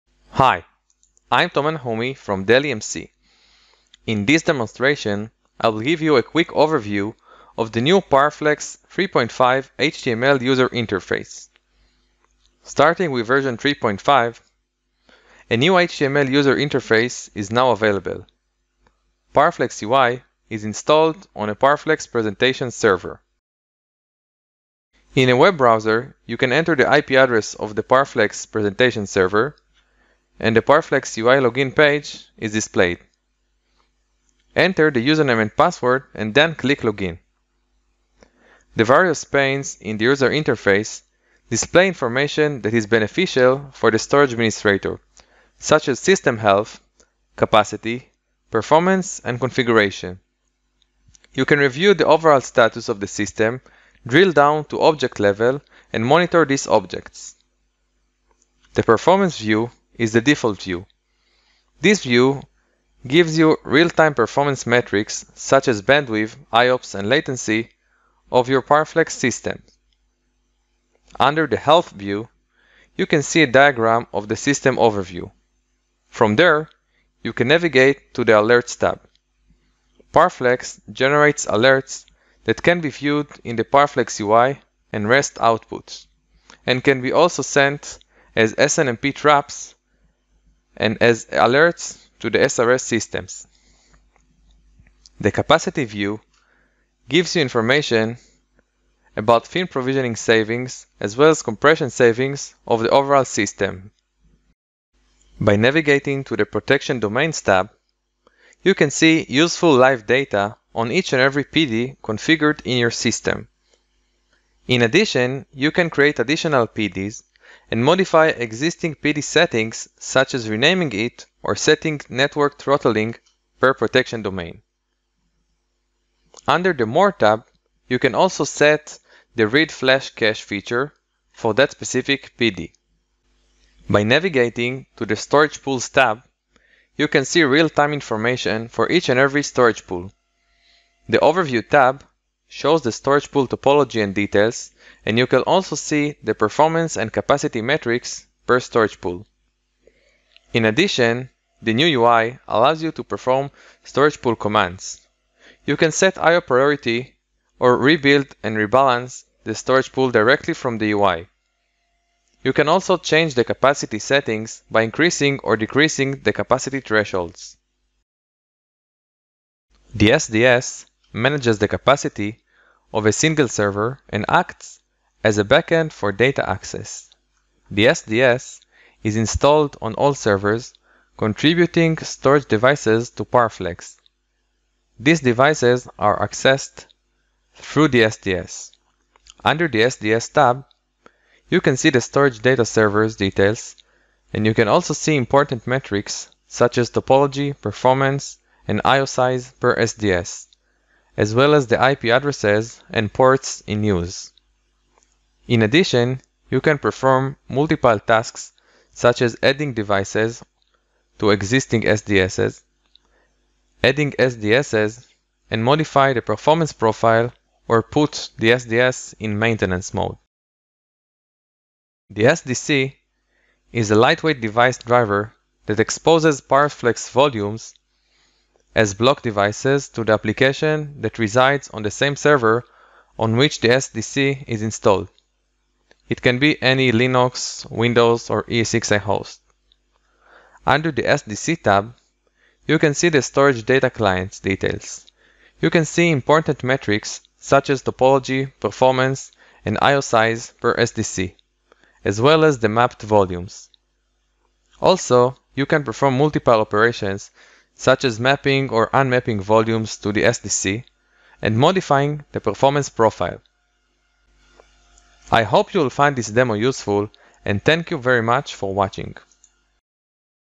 Dell EMC PowerFlex 3.5 User Interface
This demo shows the new HTML5 based, user interface of Dell EMC PowerFlex 3.5 (previously, VxFlex OS)